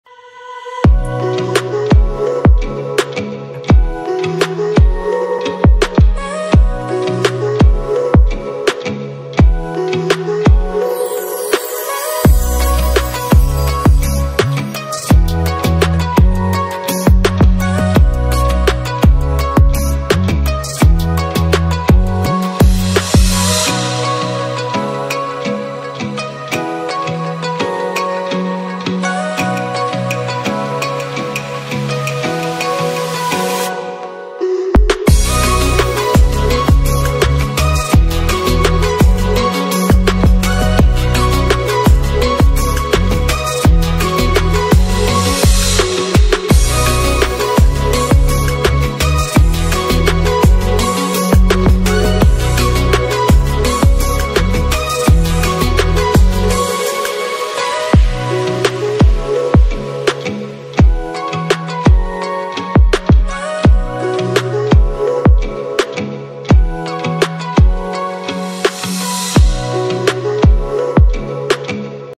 How to feed your baby Gecko| Feeding Gecko
How to feed a new born Gecko. Putting its food on the nose and he licks it.